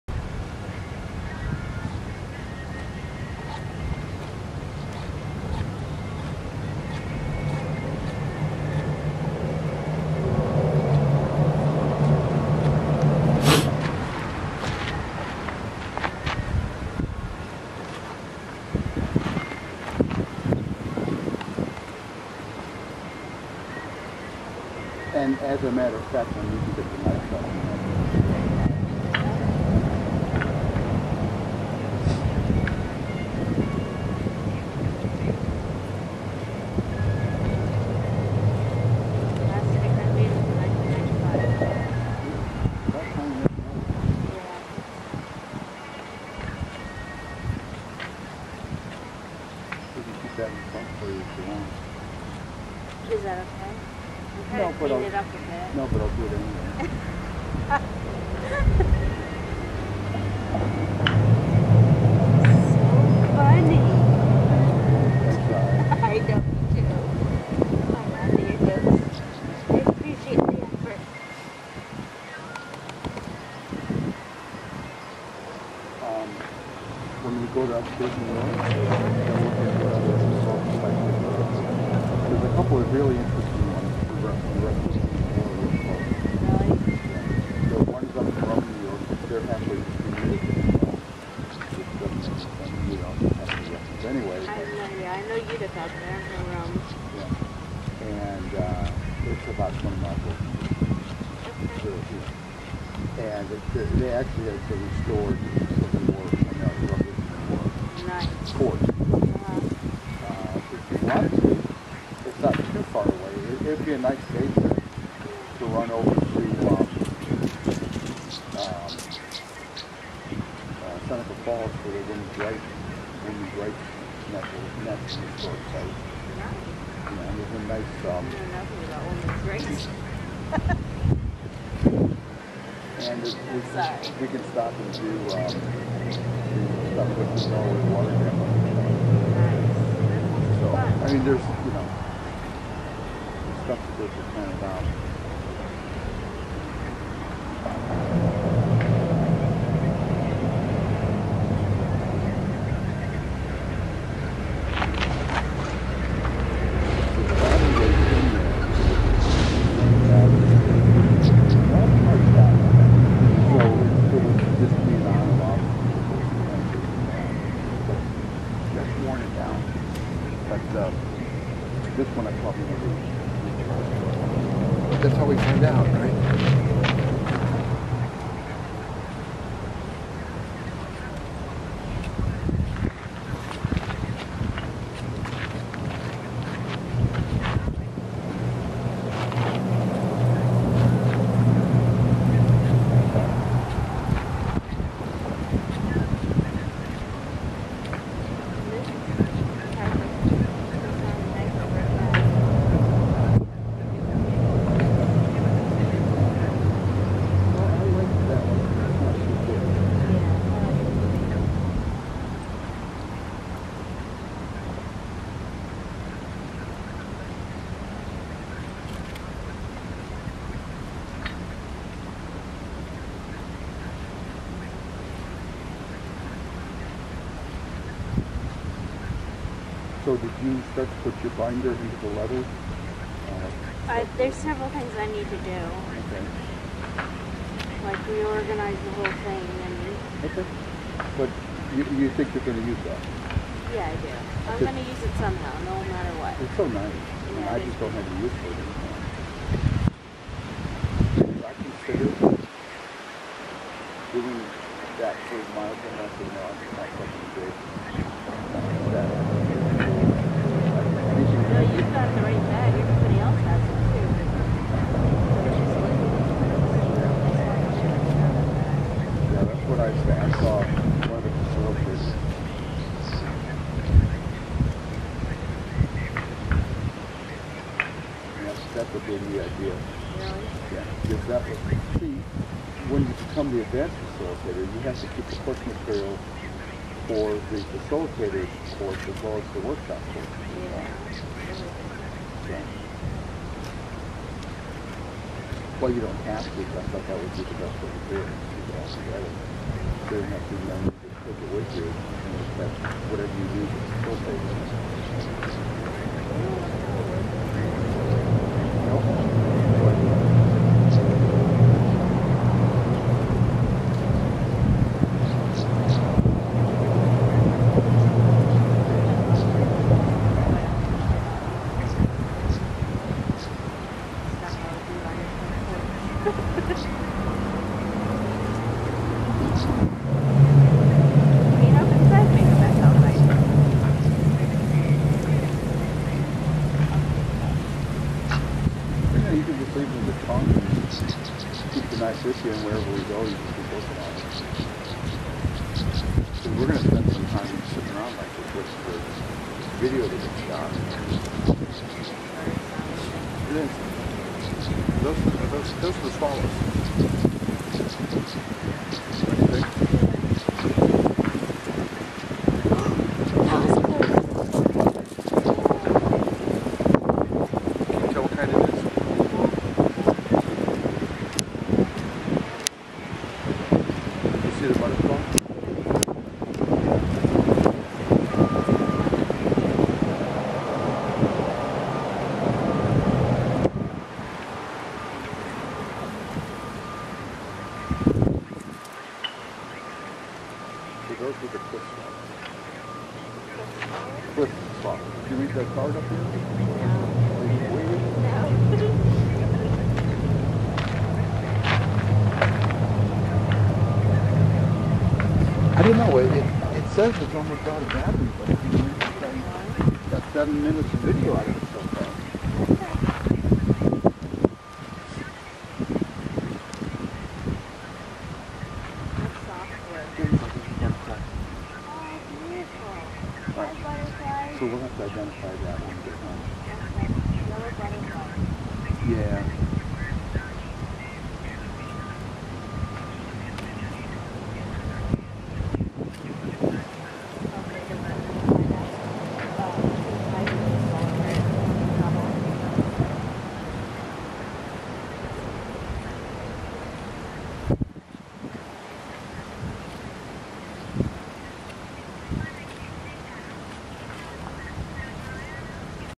Along the Delaware 04 24 2014 002
The tow path at Centre Bridge on a spring afternoon - 04/24/2014.